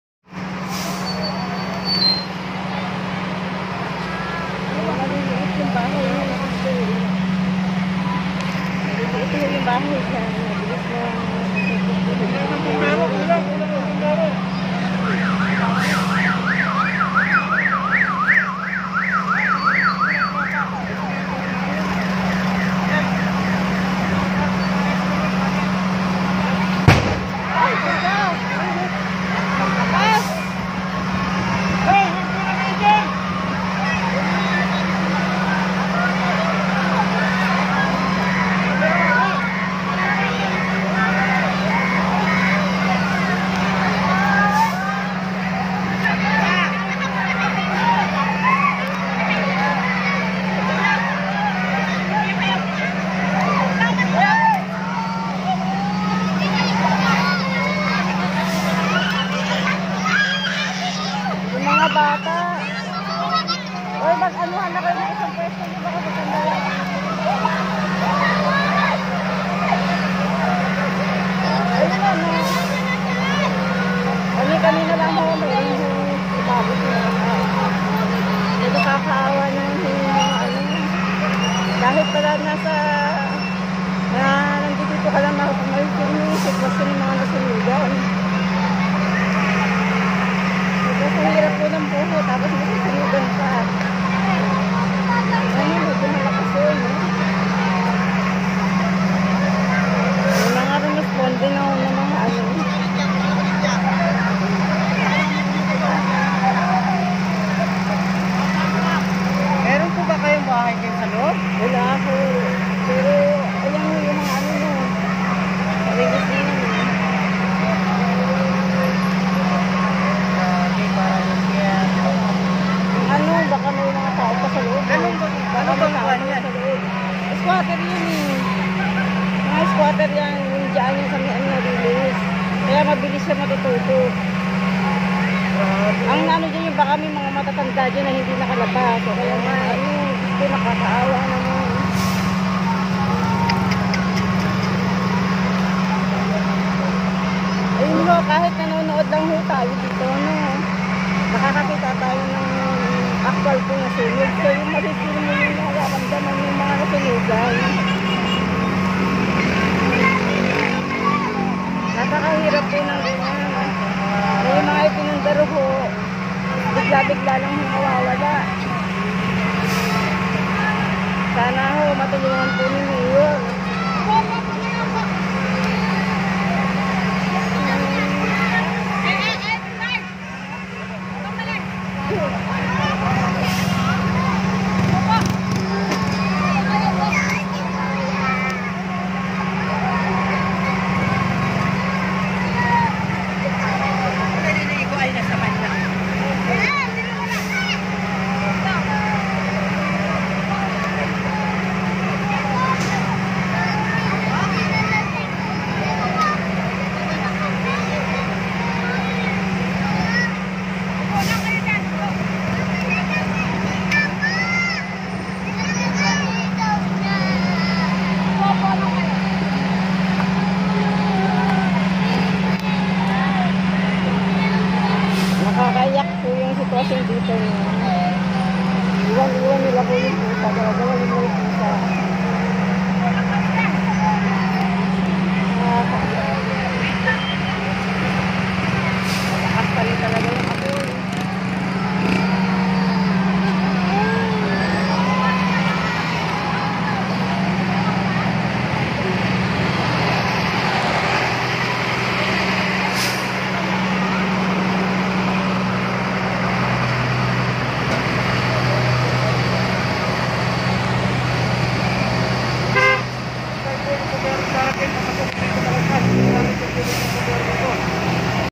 SUNOG SA SAN PEDRO LAGUNA NGAUN
SA may Chrysanthemum SA may riles Po. sobra lkas may sumabog n din